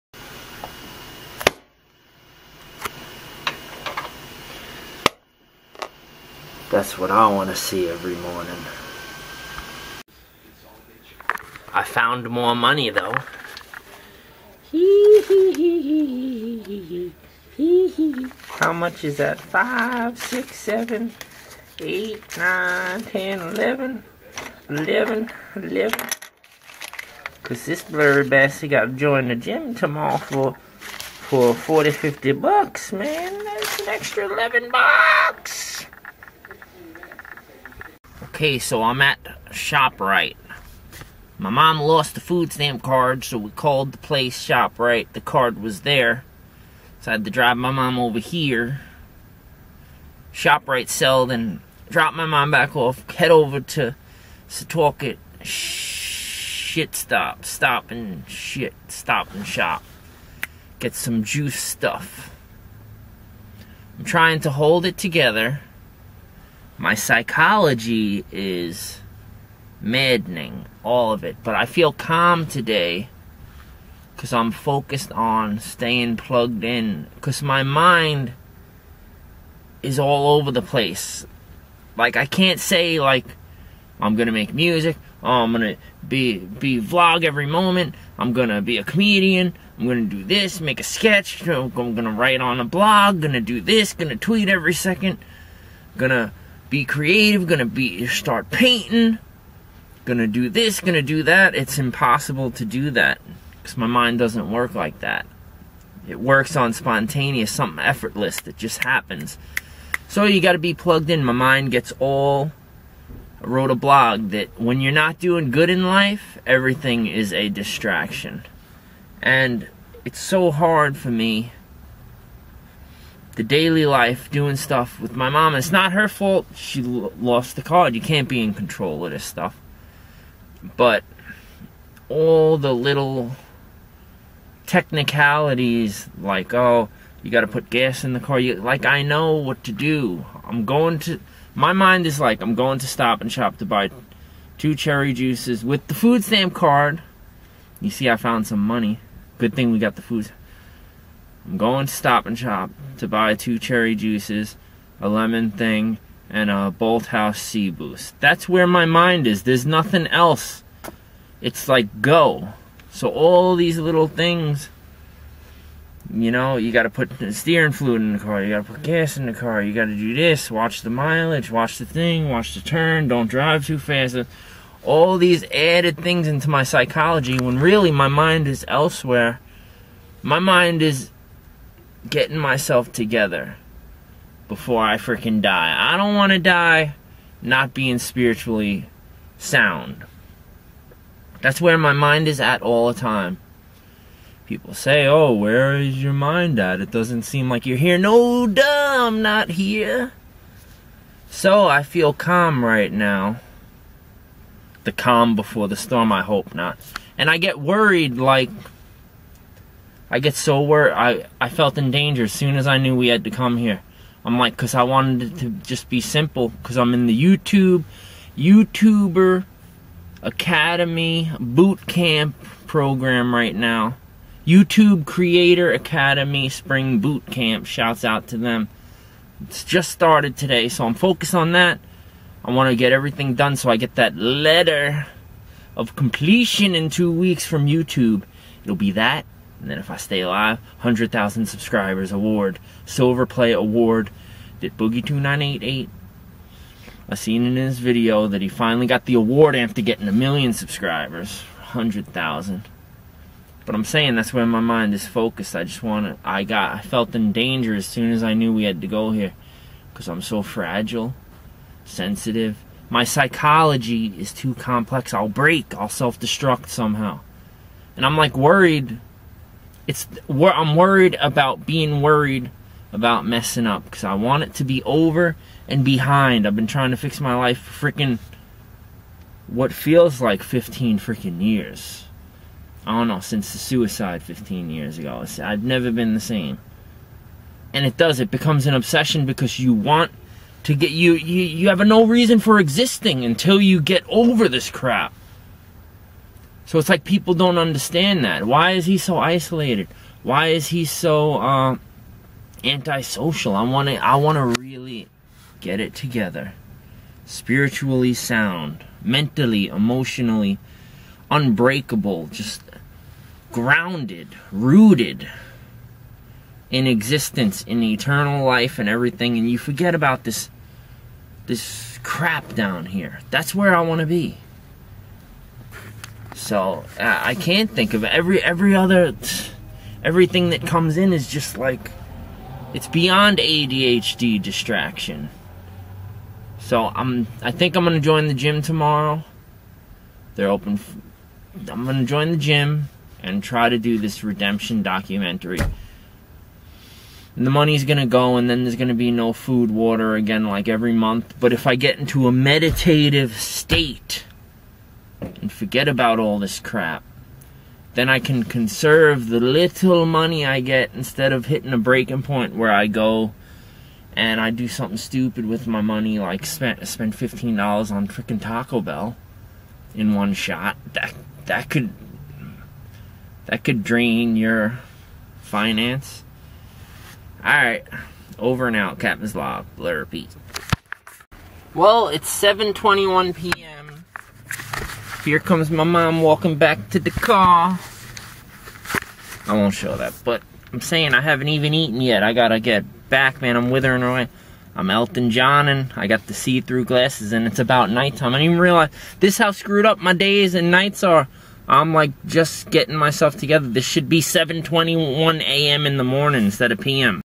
Just trying to Get thru the day
Vlog Number 910: Life has not been "the same" since suicide 15 years ago, feeling closer than ever to solving the puzzle of *SELF* and putting this mad-psychology behind me? THE SAME?